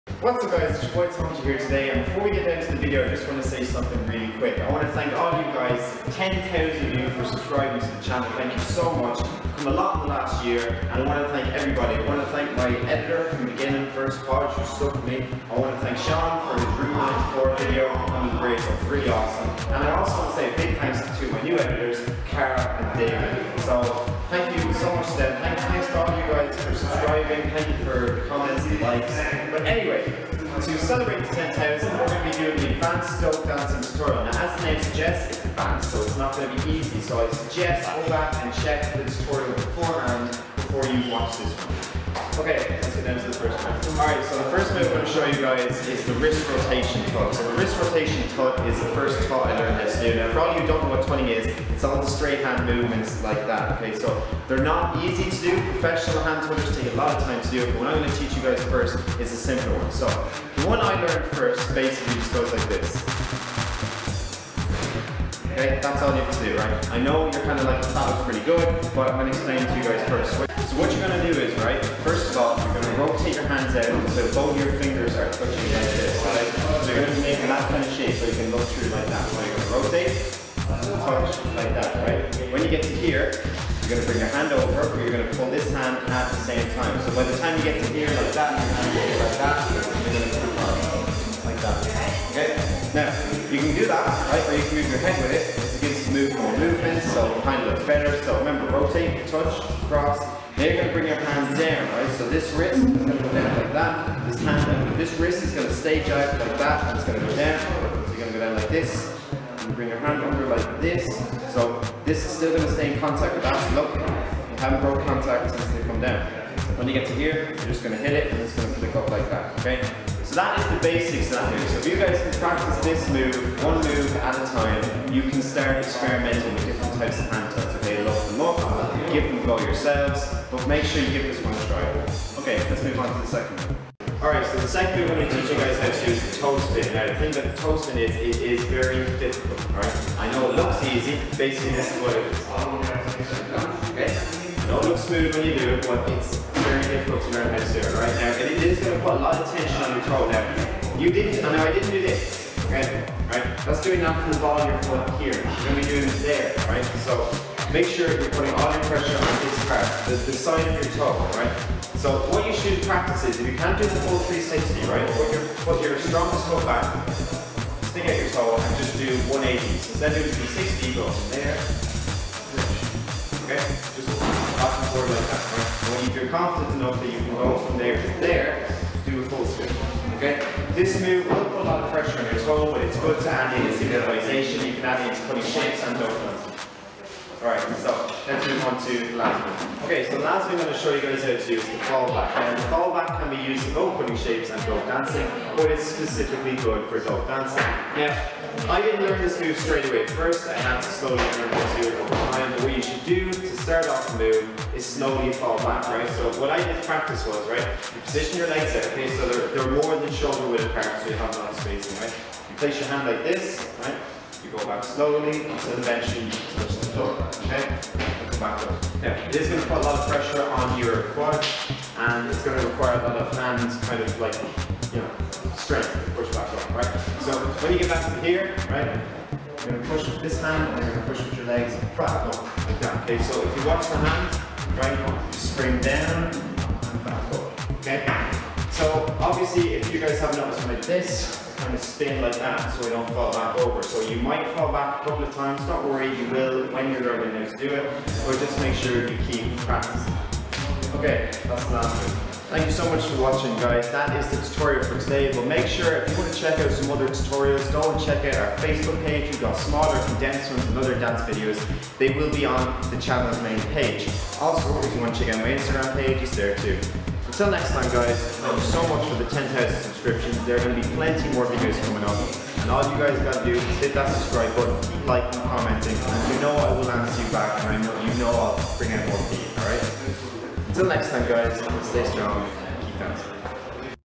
Atomic-G | Advanced Dope Dancing | Tutorial
Here we take a look at the "Wrist Rotation Tut", "The Toe Spin" and the "Fall Back".
STAY STRONG KEEP DANCING.